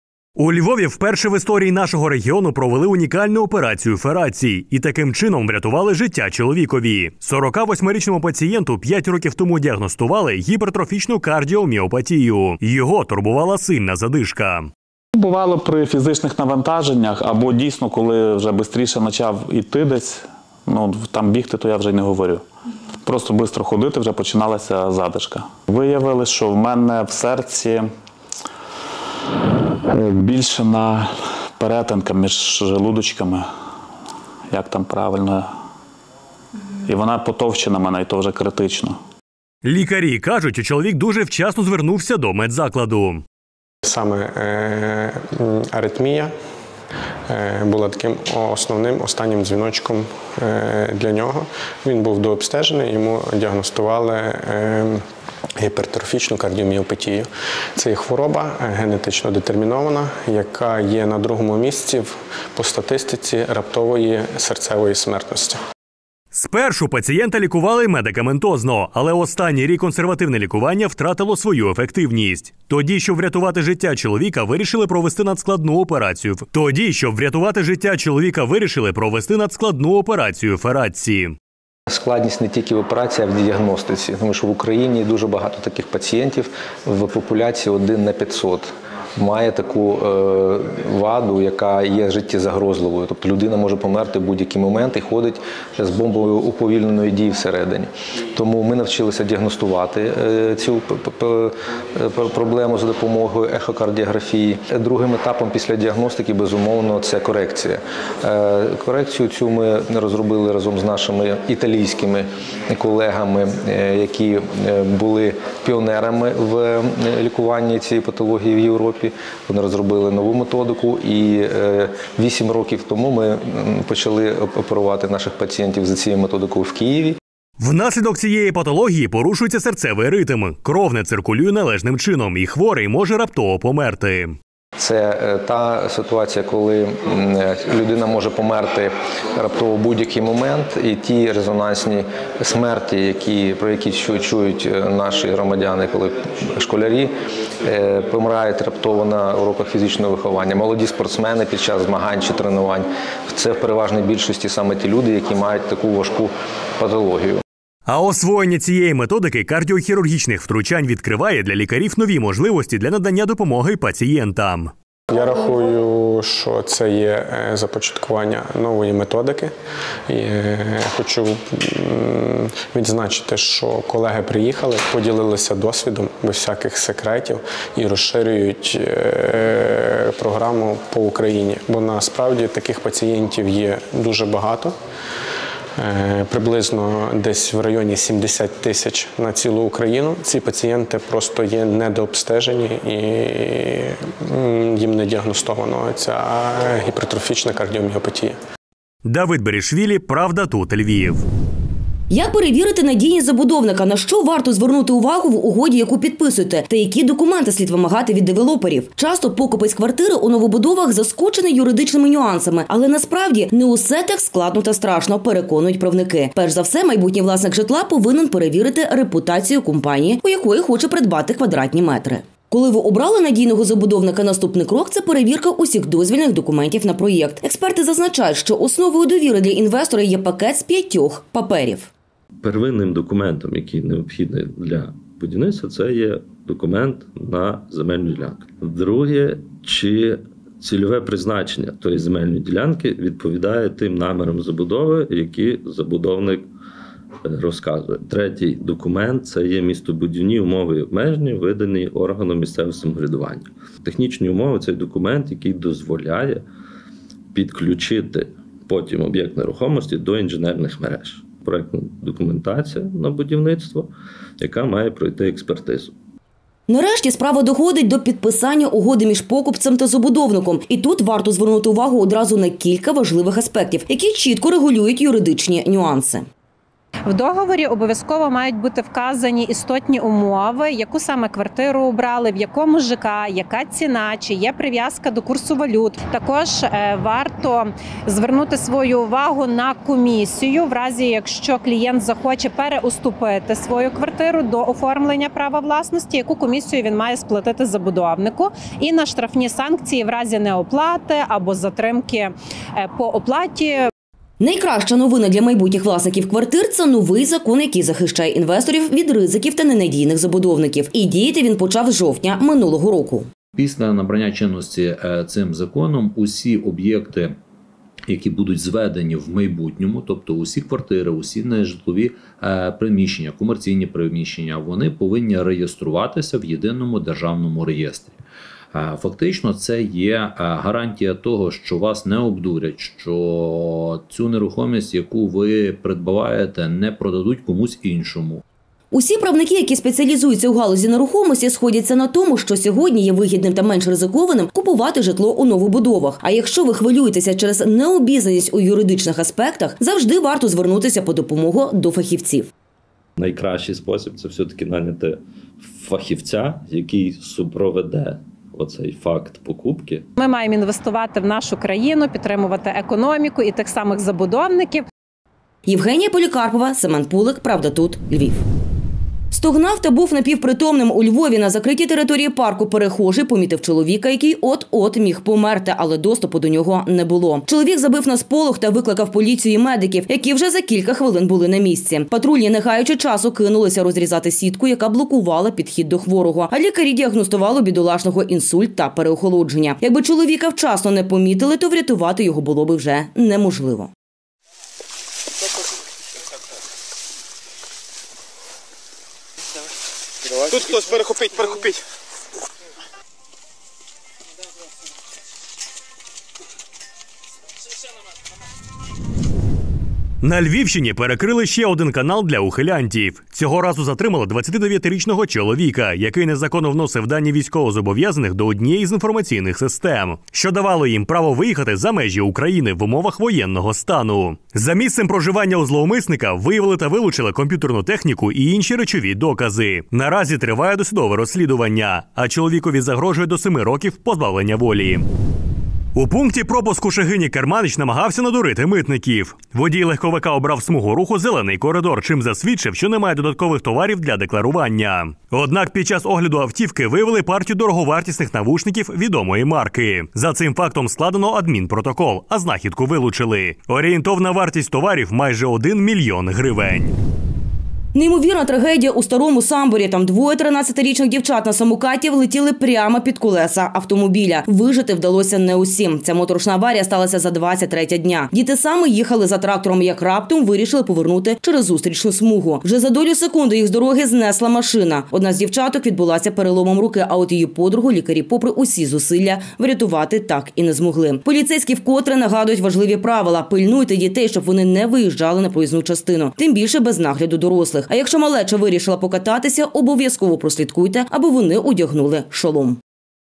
Купуємо житло: поради юристів | Унікальна операція Ферацці | Контрабанда "яблук"
У новому випуску новин на телеканалі ПравдаТУТ Львів поговоримо про те, як у пункті пропуску «Шегині» керманич намагався надурити митників. Не так усе складно та страшно – переконують правники.